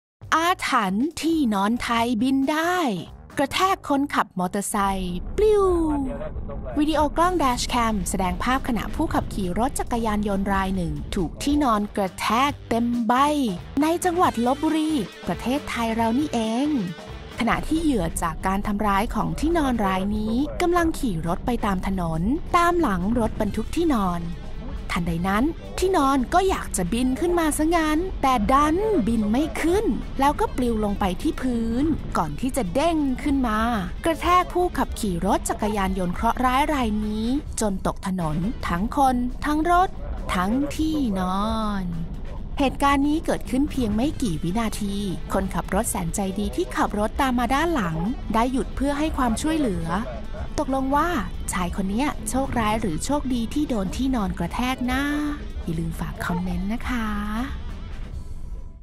อาถรรพ์ ที่นอนไทยบินได้ กระแทกคนขับมอเตอร์ไซค์ปลิว
จังหวัดลพบุรี ประเทศไทย  — วิดีโอกล้องแดชแคมแสดงภาพขณะผู้ขับขี่รถจักรยานยนต์รายหนึ่งถูกที่นอนกระแทกเต็มใบ ในจังหวัดลพบุรี ประเทศไทย

ขณะที่เหยื่อที่นอนรายนี้ กำลังขี่รถไปตามถนน ตามหลังรถบรรทุกที่นอน ทันใดนั้น ที่นอนก็อยากจะบินขึ้นมาซะงั้น แต่บินไม่ขึ้น ปลิวลงไปที่พื้น แล้วเด้งขึ้นมากระแทกผู้ขับขี่รถจักรยานยนต์เคราะห์ร้ายรายนี้ จนตกถนนทั้งคน ทั้งรถ ทั้งที่นอน

เหตุการณ์นี้นานเพียงไม่กี่วินาที คนขับรถแสนใจดีที่ตามมาด้านหลังได้หยุดให้ความช่วยเหลือ